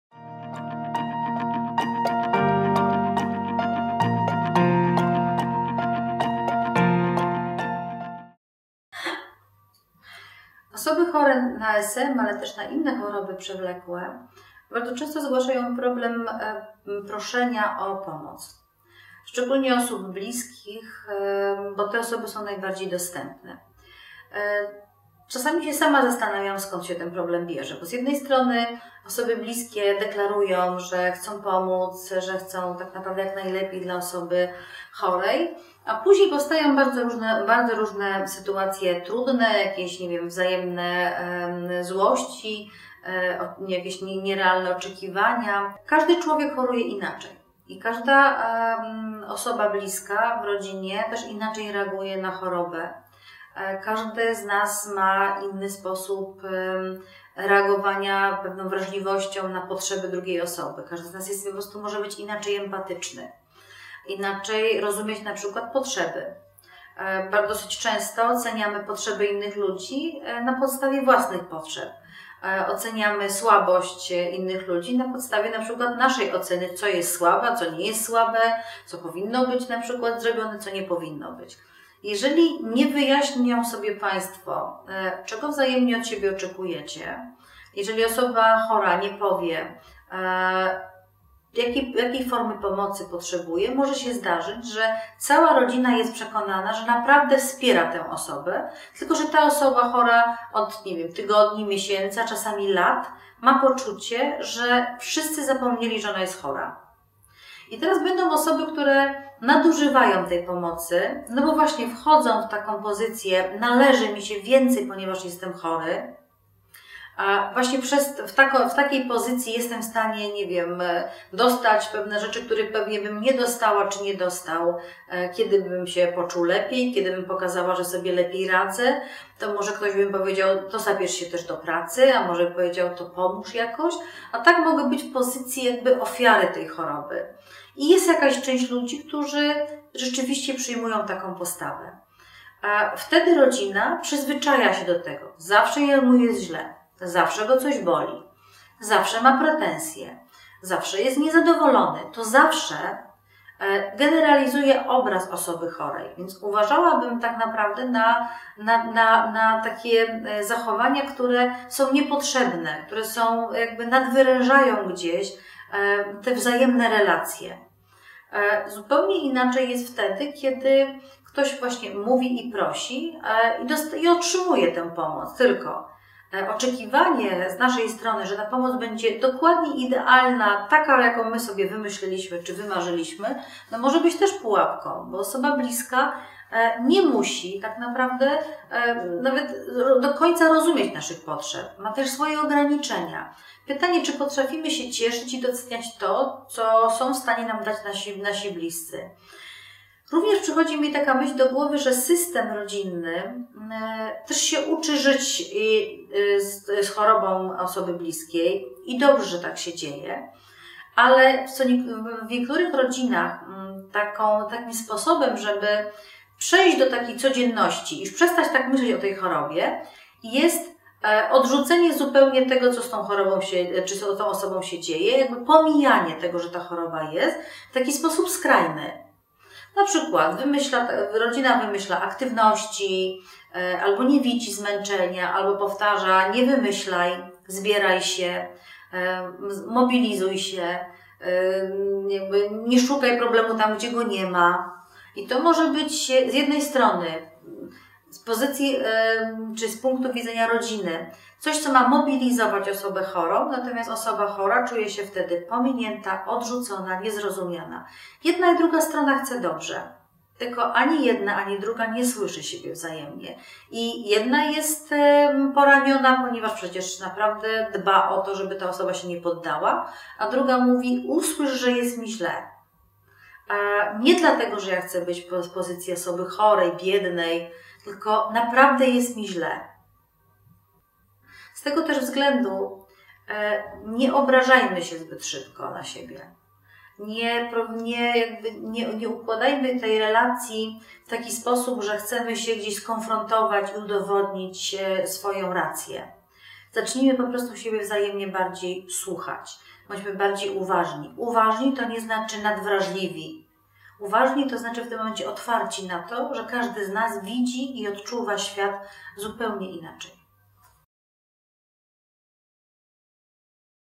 Jak prosić najbliższych o pomoc - dr Mariola Kosowicz
Zasady komunikacji z lekarzem.
Do Szkoły Motywacji zaprasza dr hab. Mariola Kosowicz - psycholog i psychoonkolog,
Od wielu lat specjalizuje się w leczeniu depresji oraz terapii par. Pracuje z pacjentami i ich rodzinami w Zakładzie Rehabilitacji w Centrum Onkologii w Warszawie (Instytut Marii Skłodowskiej-Curie). Jest terapeutką i trenerką Racjonalnej Terapii Zachowań – uczy, jak radzić sobie z poważnymi problemami osobistymi i rodzinnymi. Wspiera osoby z SM.